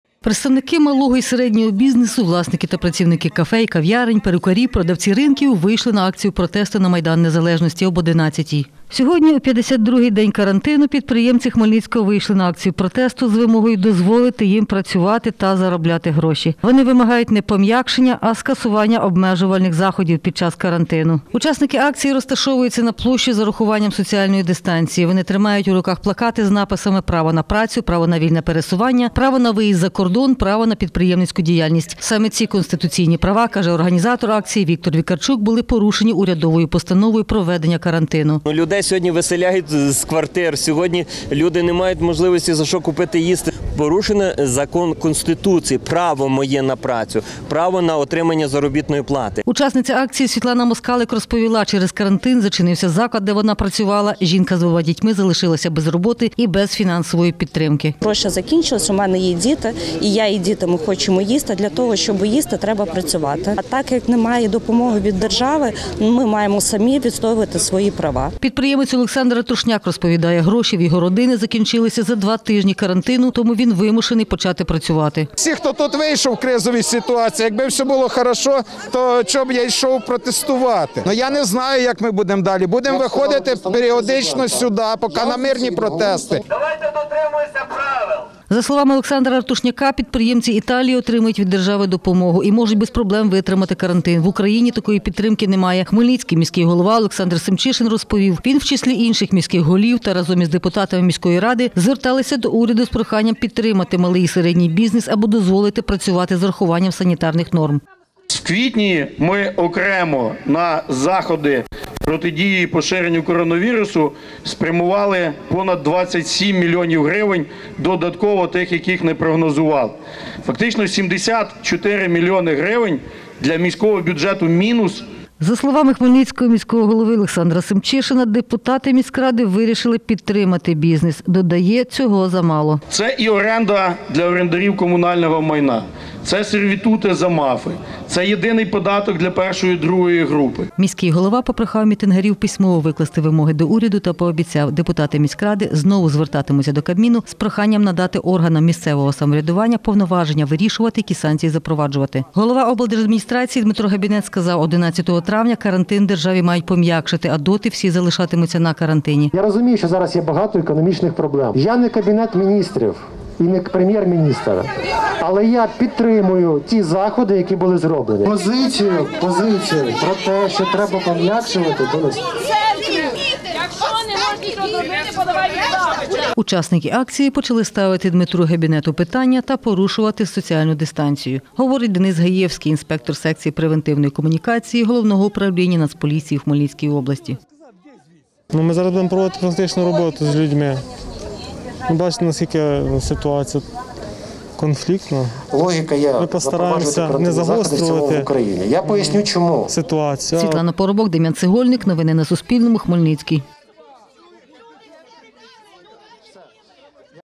Підприємці Хмельниччини вийшли на мирну акцію протесту
Близько тисячі хмельницьких підприємців сьогодні, 4 травня, вийшли на мирну акцію з вимогою дозволити їм працювати. Мітингарі - представники малого й середнього бізнесу, власники та працівники кафе й кав’ярень, перукарі, продавці ринків.

Учасники тримали в руках плакати з написами: «Право на працю», «Право на вільне пересування», «Право на виїзд за кордон», «Право на підприємницьку діяльність». Саме ці конституційні права, каже організатор акції Віктор Вікарчук, порушені урядовою постановою про введення карантину, і дію цієї постанови мітингарі хочуть скасувати.